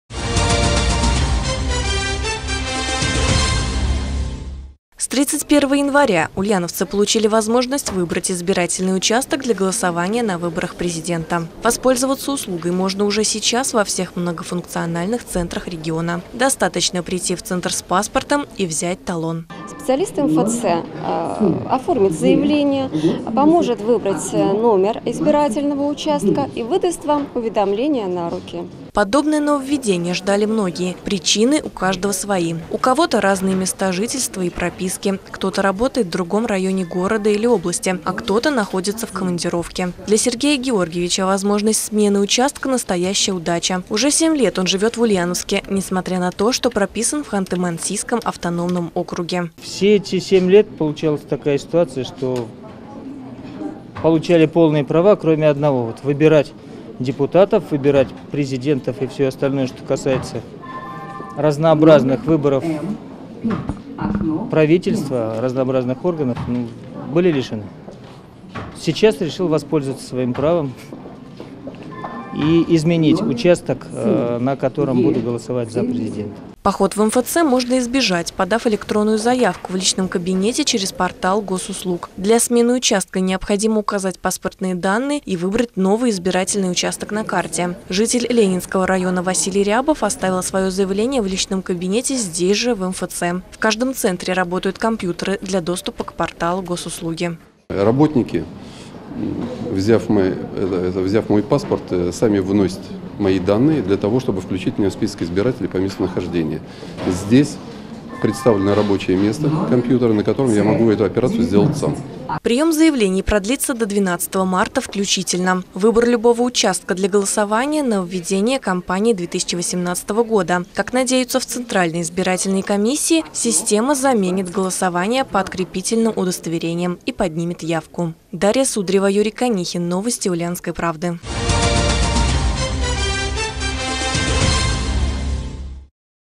Через Госуслуги или МФЦ. В Ульяновске стартовал прием заявок на смену места голосования
С 31-го января ульяновцы получили возможность выбрать избирательный участок для голосования на выборах президента. Воспользоваться услугой можно уже сейчас во всех многофункциональных центрах региона.